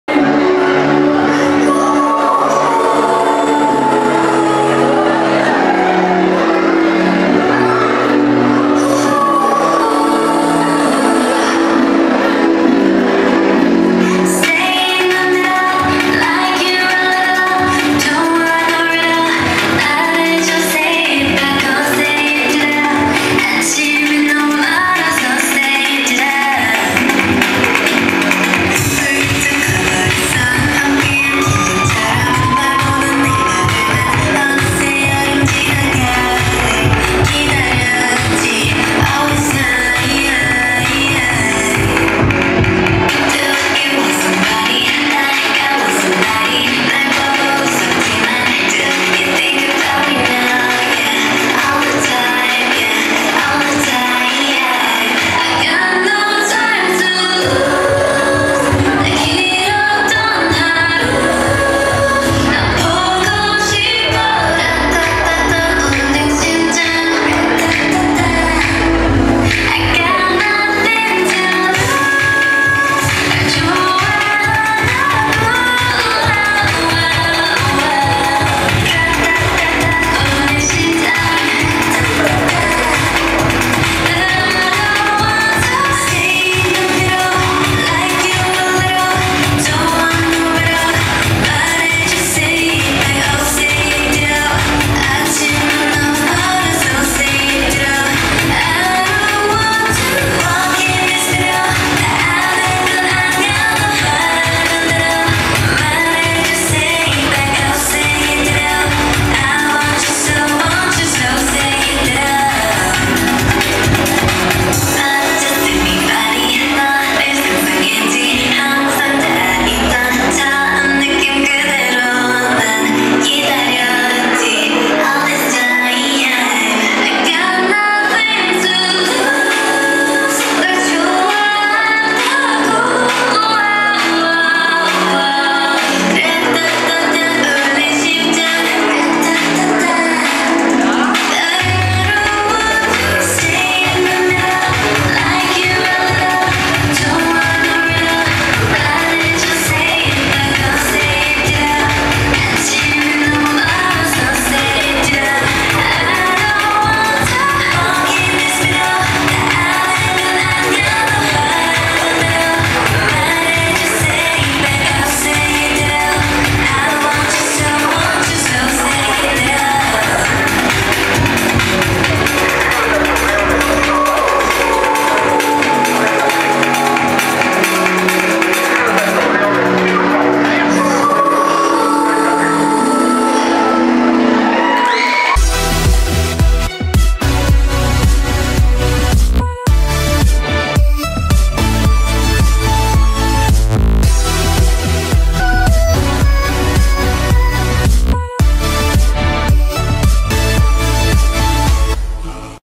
MILKA : Presentación en SHIMAI 2023 - 🇲🇽 Cancún, México. | K-POP Dance Cover.
¿Ya sigues nuestros otros canales?